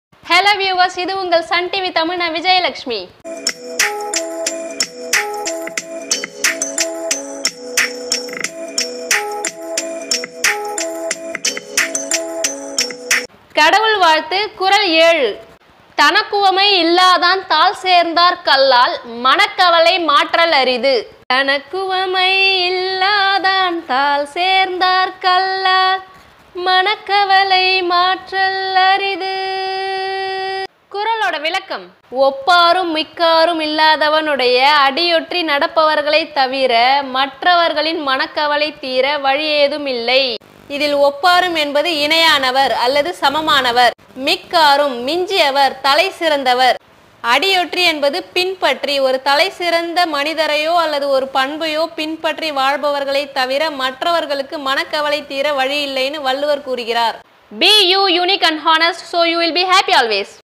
Thirukkural_அதிகாரம் 1_கடவுள் வாழ்த்து_குறள் 7_தனக்குவமை இல்லாதான்_Thiruvalluvar_Son TV Tamil
திருக்குறள்

அதிகாரம் : 1

கடவுள் வாழ்த்து

குறள் : 7

தனக்குவமை இல்லாதான் தாள்சேர்ந்தார்க் கல்லால்
மனக்கவலை மாற்றல் அரிது

விளக்கம்:

ஒப்பாரும் மிக்காருமில்லாதவனுடைய அடியொற்றி நடப்பவர்களைத் தவிர, மற்றவர்களின் மனக்கவலை தீர வழியேதுமில்லை.

English meaning :

Anxiety of mind cannot be removed, except from those who are united to the feet of Him who is incomparable.

Give your valuable feedback below in the comments session.✍️✍️✍️

Thanks for spending your precious time to watch my video❤️❤️❤️❤️

நன்றி🙏